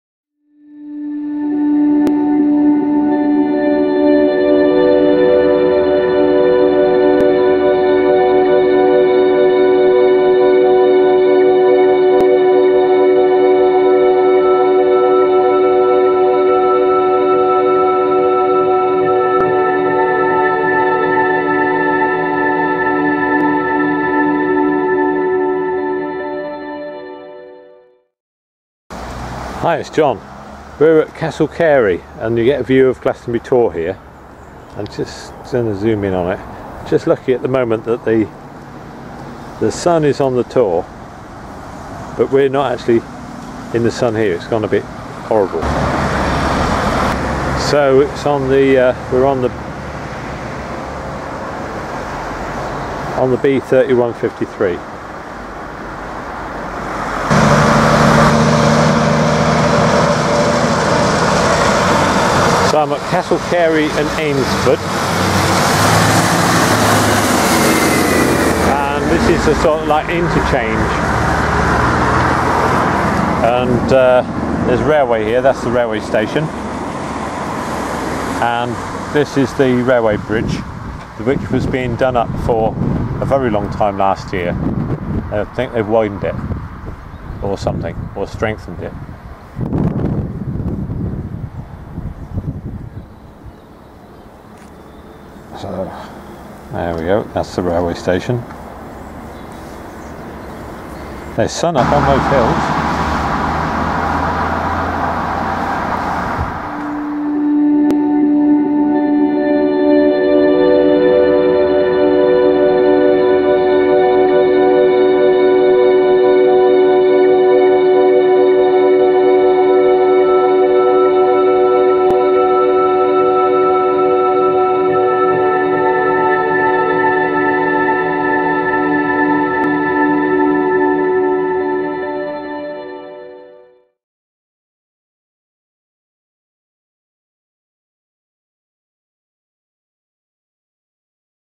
Glastonbury Tor filmed from Castle Cary
Glastonbury Tor has a special mysticism about it - it is connected to the Arthurian Legends and holds a fascination for many people.